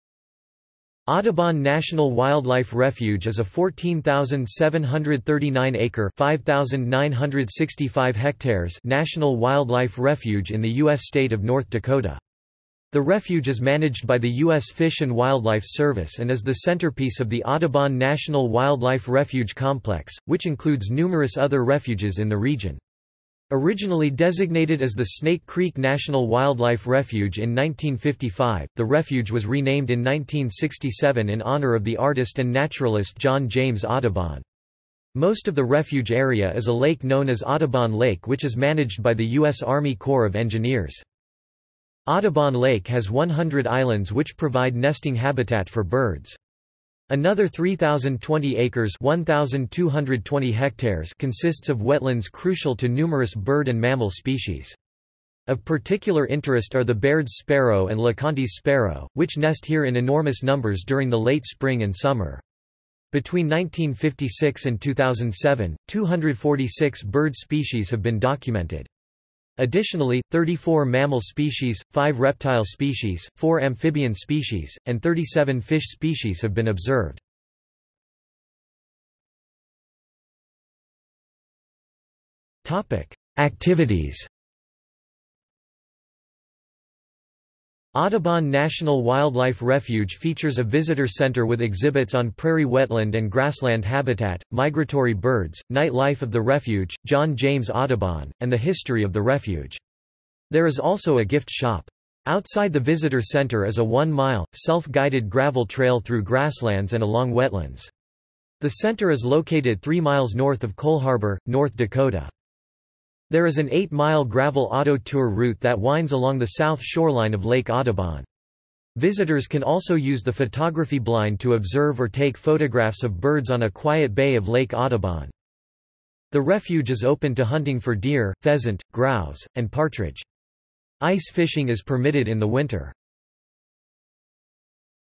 Audubon National Wildlife Refuge | Wikipedia audio article
This is an audio version of the Wikipedia Article:
Audubon National Wildlife Refuge

00:01:19 undefined

- Socrates

SUMMARY
Audubon National Wildlife Refuge is a 14,739-acre (5,965 ha) National Wildlife Refuge in the U.S. state of North Dakota. The refuge is managed by the U.S. Fish and Wildlife Service and is the centerpiece of the Audubon National Wildlife Refuge Complex, which includes numerous other refuges in the region. Originally designated as the Snake Creek National Wildlife Refuge in 1955, the refuge was renamed in 1967 in honor of the artist and naturalist John James Audubon. Most of the refuge area is a lake known as Audubon Lake which is managed by the U.S. Army Corps of Engineers.
Audubon Lake has one hundred islands which provide nesting habitat for birds. Another 3,020 acres (1,220 ha) consists of wetlands crucial to numerous bird and mammal species. Of particular interest are the Baird's sparrow and Le Conte's sparrow, which nest here in enormous numbers during the late spring and summer. Between 1956 and 2007, 246 bird species have been documented. Additionally, 34 mammal species, five reptile species, four amphibian species, and 37 fish species have been observed.